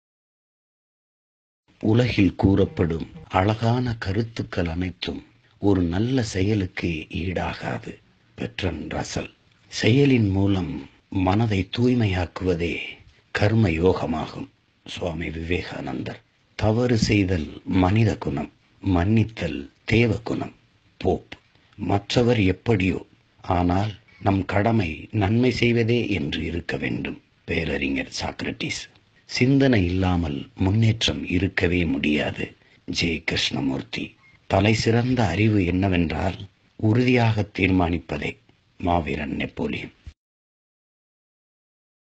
13-03-2018 நாளும் பல நற்செய்திகள் - 275 | பகுதி - 02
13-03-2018 நாளும் பல நற்செய்திகள் - 275 | பகுதி - 02

உலகில் கூறப்படும் அழகான கருத்துக்கள் அனைத்தும் ஒரு நல்ல செயலுக்கு ஈடாகாது!
- பெட்ரன் ரசல்

செவுளின் மூலம் மனதை தூய்மையாக்குவதே கர்மயோகமாகும்!
- சுவாமி விவேகானந்தர்

தவறு செய்தல் மனித குணம்! மன்னித்தல் தேவ குணம்!
- போப்

மற்றவர் எப்படியோ ஆனால் நம் கடமை நன்மை செய்வதே என்று இருக்க வேண்டும்!
- பேரறிஞர் சாக்ரடிஸ்

சிந்தனை இல்லாமல் முன்னேற்றம் இருக்கவே முடியாது!
- ஜே. கிருஷ்ணமூர்த்தி

தலைசிறந்த அறிவு என்னவென்றால் உறுதியாகத் தீர்மானிப்பதே!
- மாவீரன் நெப்போலியன்

சீமான்
தலைமை ஒருங்கிணைப்பாளர்